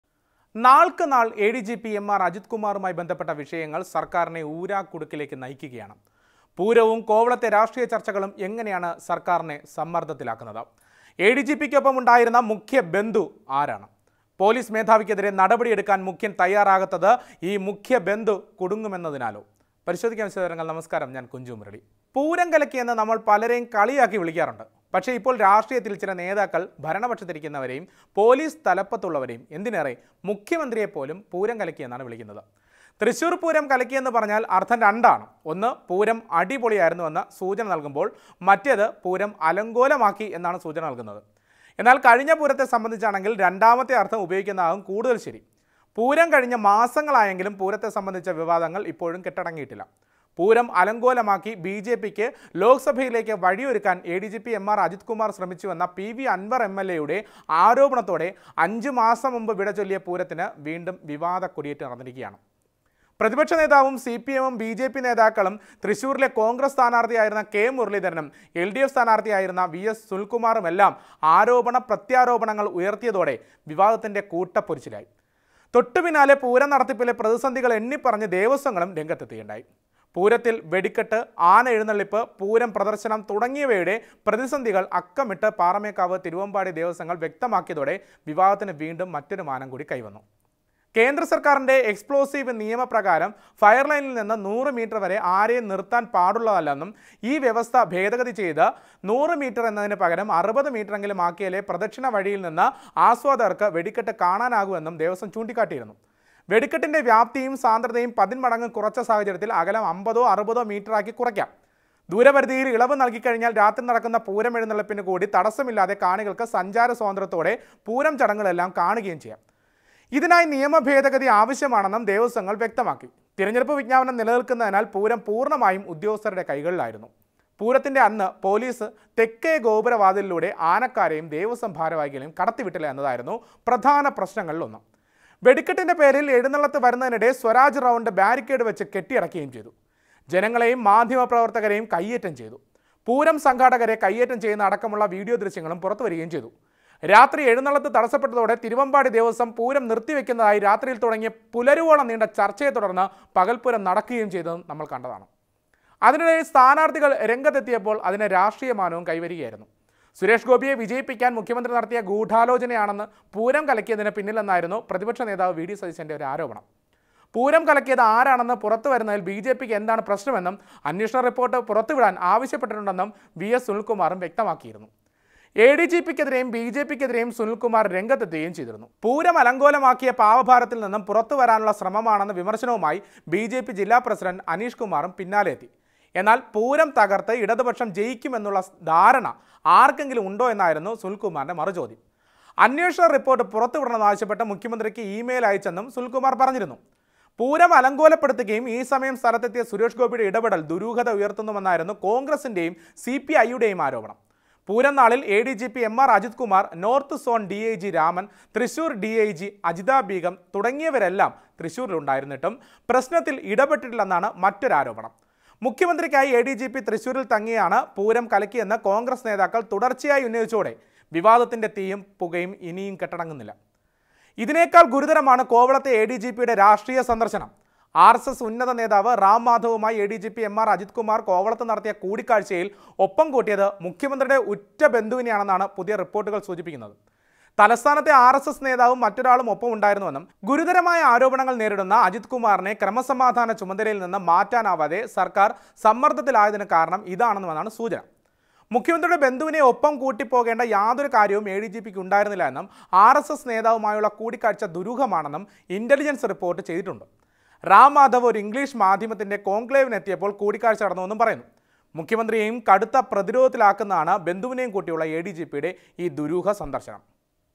'മുഖ്യ ബന്ധു'വിനെ രക്ഷിക്കാന്‍ എഡിജിപിയെ രക്ഷിച്ച് മുഖ്യന്‍?| CM Pinarayi Vijayan | ADGP Ajith Kumar
നാള്‍ക്കുനാള്‍ എഡിജിപി എം.ആര്‍ അജിത് കുമാറുമായി ബന്ധപ്പെട്ട വിഷയങ്ങള്‍ സര്‍ക്കാരിനെ ഊരാക്കുടുക്കിലേക്ക് നയിക്കുകയാണ്. പൂരവും കോവളത്തെ രാഷ്ട്രീയ ചര്‍ച്ചകളും എങ്ങനെയാണ് സര്‍ക്കാരിനെ സമ്മര്‍ദ്ദത്തിലാക്കുന്നത്? എ ഡി ജി പിക്കൊപ്പം ഉണ്ടായിരുന്ന മുഖ്യ ബന്ധു ആരാണ്? പോലീസ് മേധാവിക്കെതിരെ നടപടിയെടുക്കാന്‍ മുഖ്യന്‍ തയ്യറാകാത്ത ഈ മുഖ്യ ബന്ധു കുടുങ്ങുമെന്നതിനാലോ?